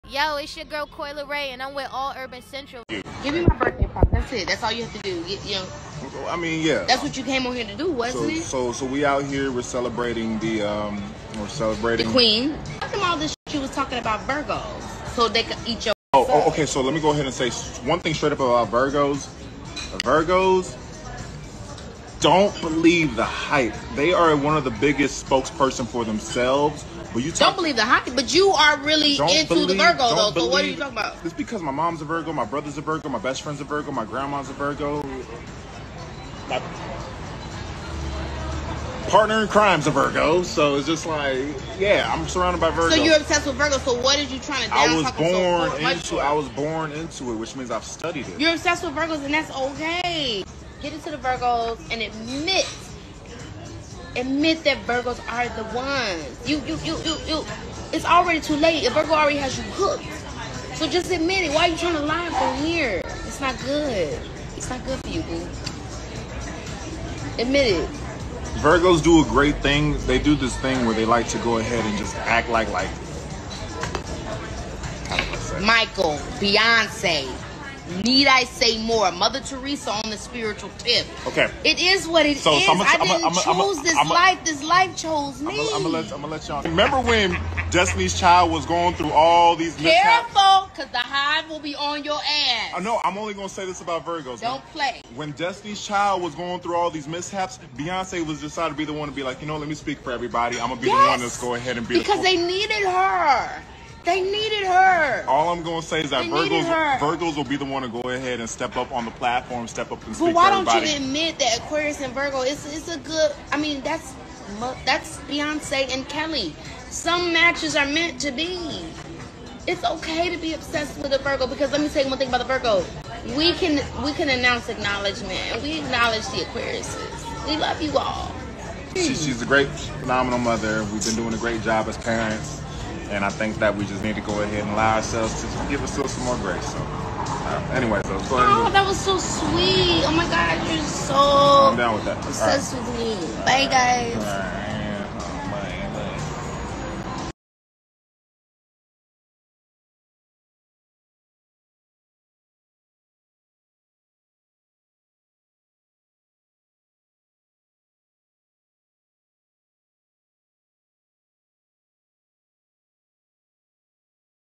"I Got Him Hooked" Keke Palmer Spends Bday With Baby Daddy Darius Jackson Amidst Breakup Over Usher
Keke Palmer And Darius Jackson Still Going Strong After Breakup Rumors Goes Live On Her Birthday